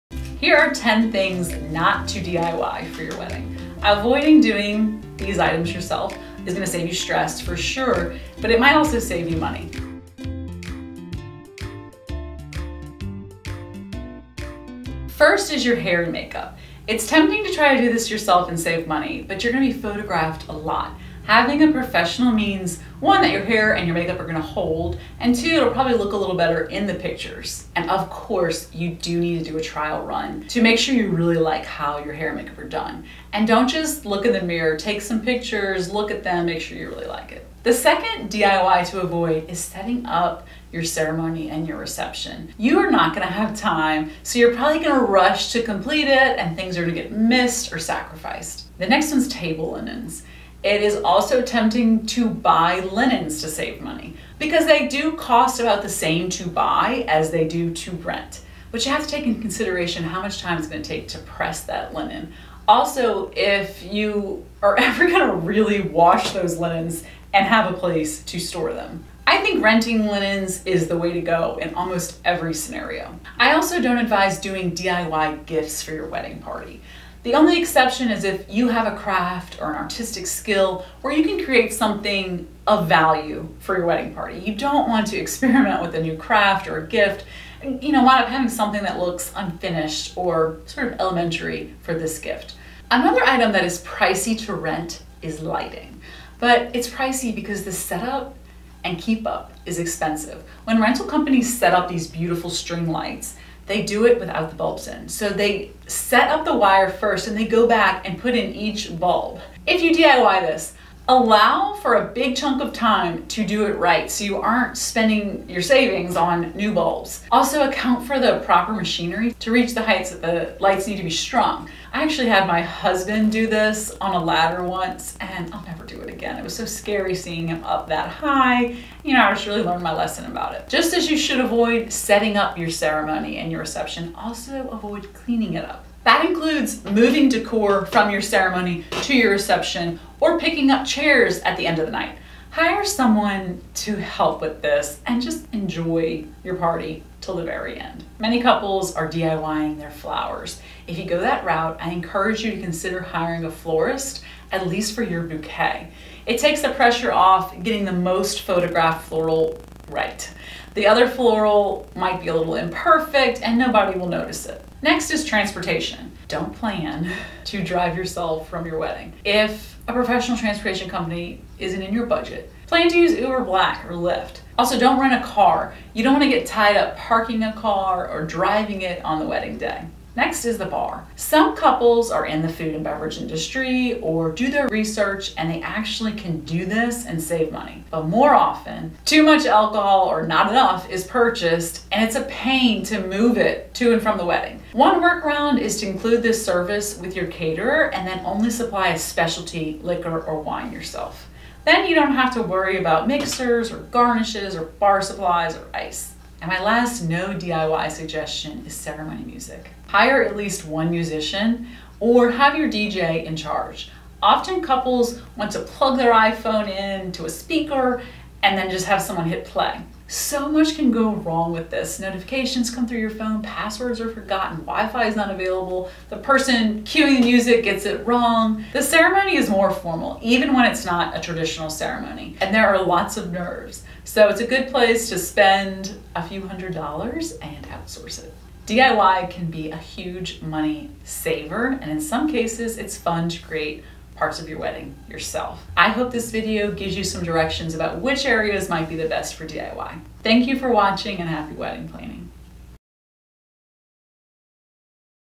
10 Things NOT to DIY for Your Wedding
Here are 10 Things not to DIY for your wedding. Avoiding doing these items yourself, will save you stress for sure, and possibly money in reality.

1. Your Hair and Makeup
2. Setting up your ceremony and reception
3. Table linens
4. Gifts for your wedding party
5. Lighting
6. Cleaning up your ceremony and reception
7. Flowers
8. Transportation
9. Bar
10. Ceremony Music

DIY can be a huge money saver and in some cases, it's fun to create parts of your wedding yourself. I hope this video gives you some direction about which areas might be best for DIY.